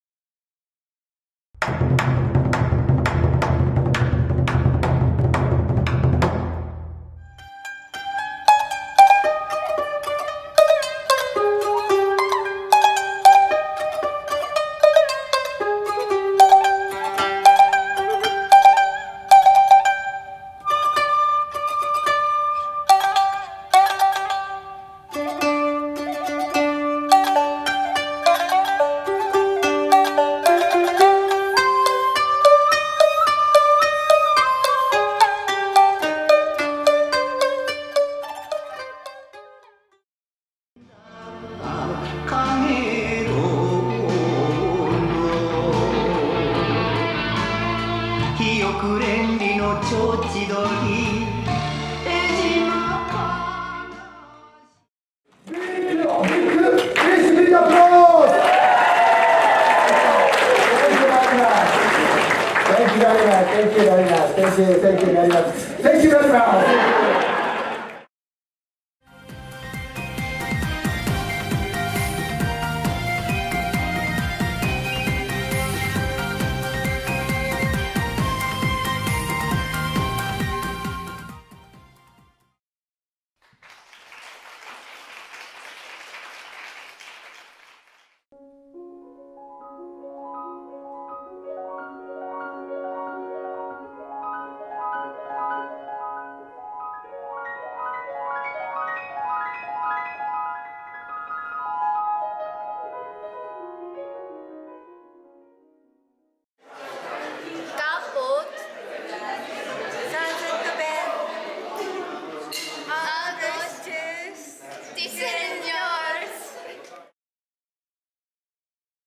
KAPOCS 150
Japán vendégművészekkel egész napos program a Déri Múzeumban 2019. augusztus 18-án.